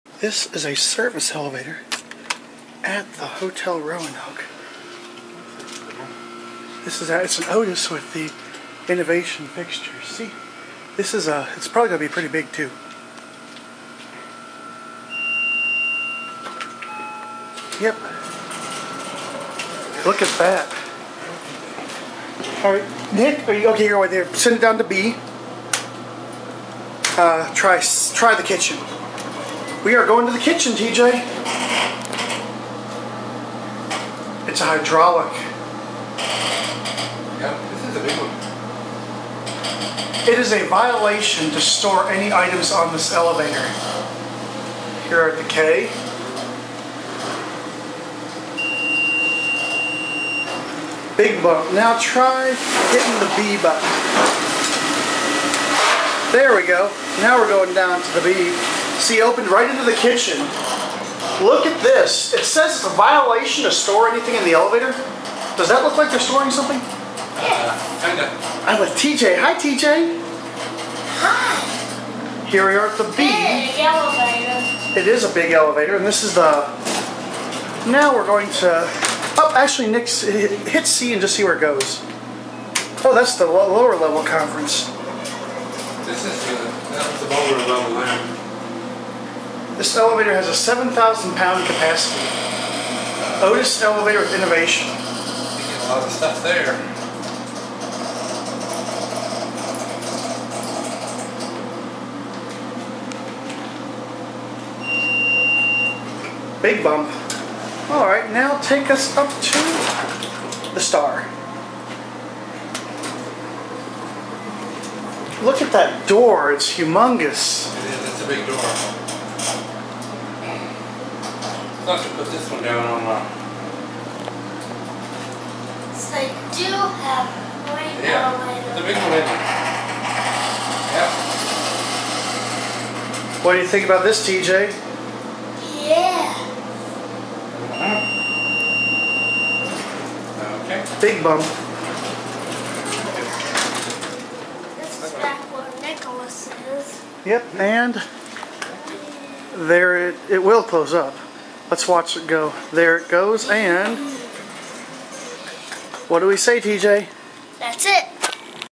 TJelevatorfan rides the freight elevator at the hotel Roanoke
This is from TJ's first trip. he REALLY enjoyed himself! Facebook